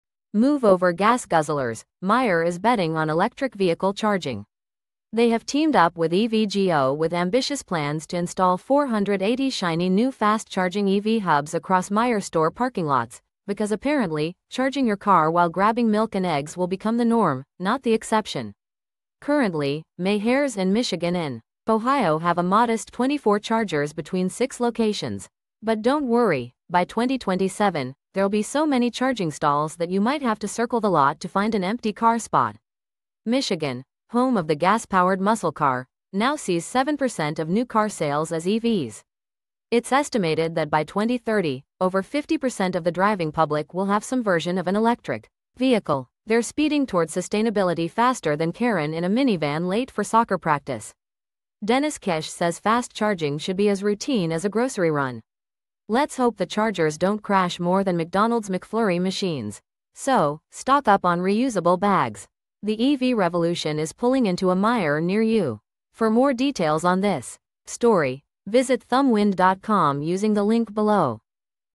Forget Gas Stations—Your Local Meijer Is Looking To Be The New EV Charging Hub In Michigan
EVgo and Meijer team up to bring up to 480 fast-charging stalls to Midwest grocery stores, fueling Michigan’s EV charging hub revolution with a sustainable future in mind.

Michigan’s Grocery Stores to Host EV Fast Charging Revolution
EVgo Inc., one of the largest public fast charging networks in the U.S., and Meijer, a leading Midwest grocery chain, are accelerating Michigan’s transition to electric vehicles (EVs). Announced this week, the partnership will deploy up to 480 new fast charging stalls at Meijer properties across Michigan and five other states, solidifying the region’s infrastructure for EV growth.